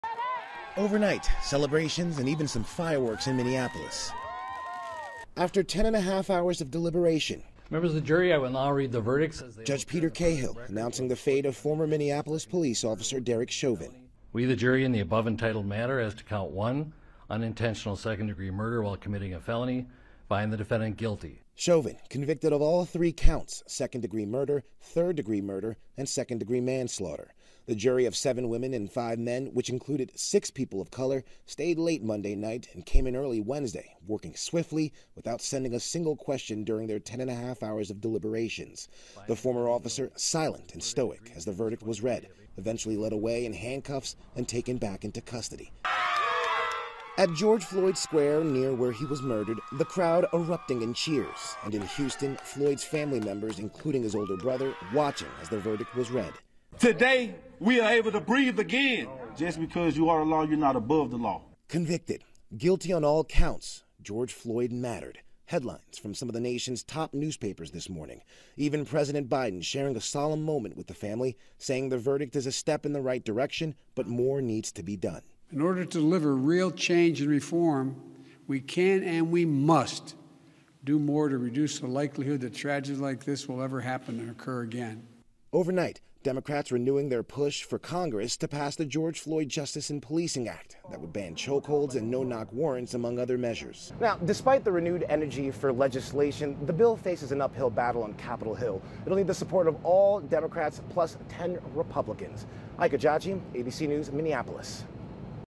Derek Chauvin convicted on all 3 counts by jury of his peers
President Biden says the verdict is a step in the right direction.